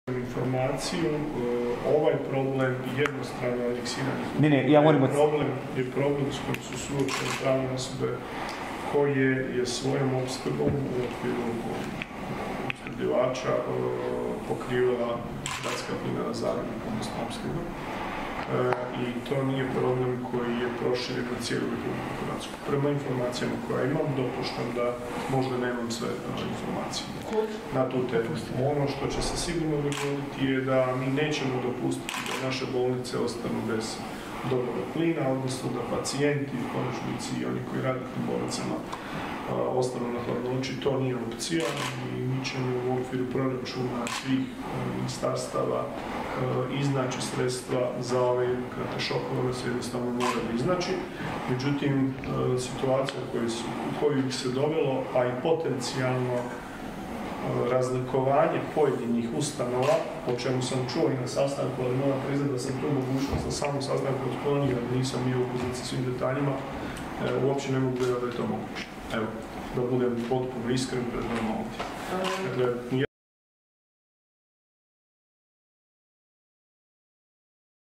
Tomislav Ćorić: Mi nećemo dopustiti da naše bolnice ostanu bez plina
Nakon sastanka vladajuće koalicije ministar gospodarstva i održivog razvoja Tomislav Ćorić dao je izjavu o opskrbi plinom hrvatskih bolnica.